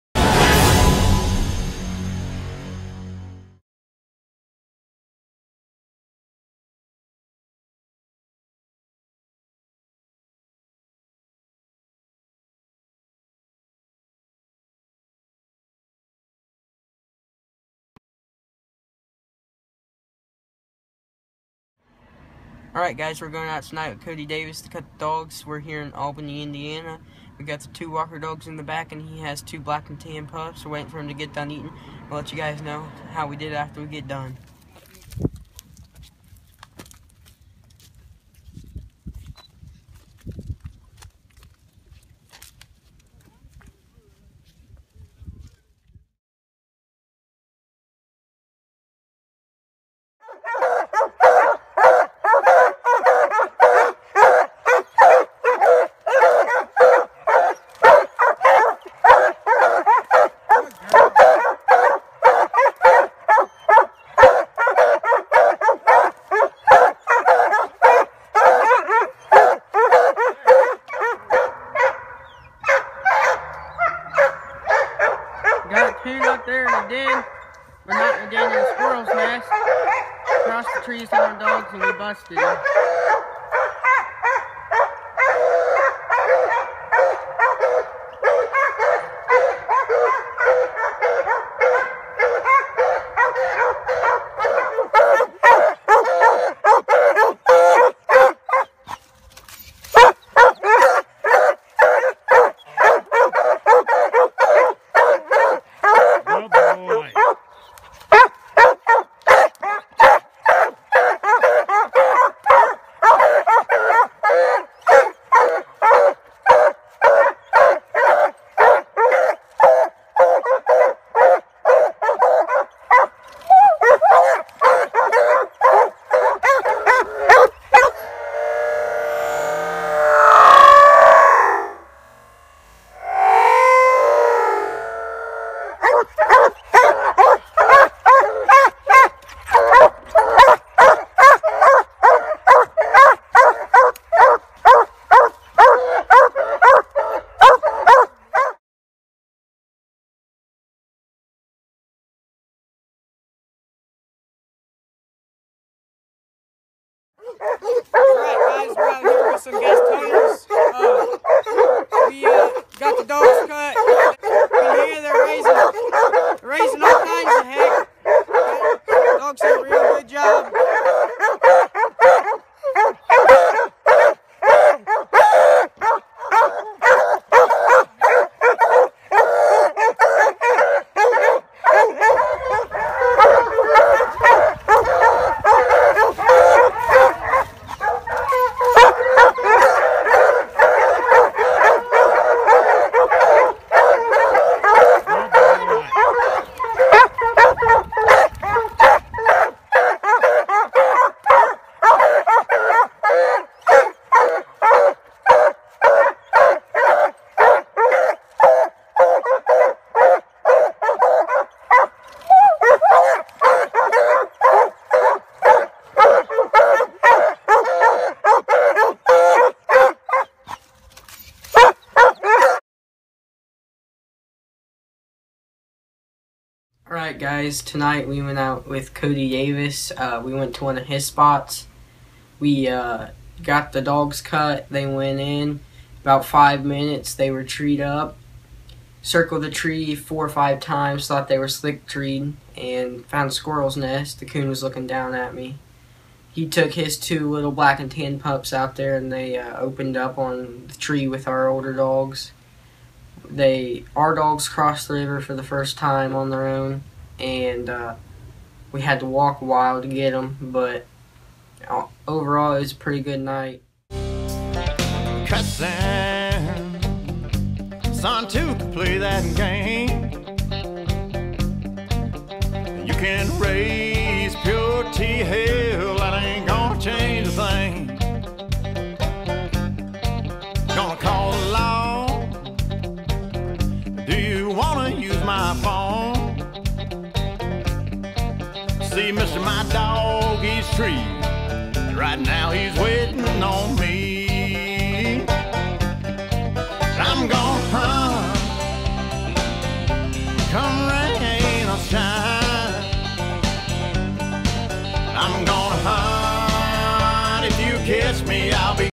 Team Lil'Cooners Coon hunting with Black & Tans
Team Lil'Cooners helping Cody Davis train B&T pups don't forget to check us out on FB Instagram @carcass makers and on Goggle+